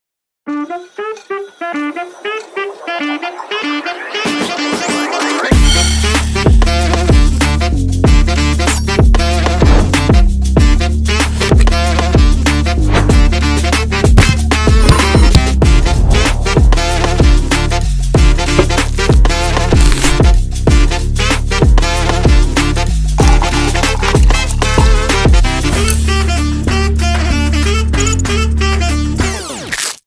Daniel Schiffer x Storyblocks Editing Challenge 2022｜by Peter
this is My entry into the Daniel Schiffer x Storyblocks editing challenge.
This is a very interesting activity, I'm very honored to be able to participate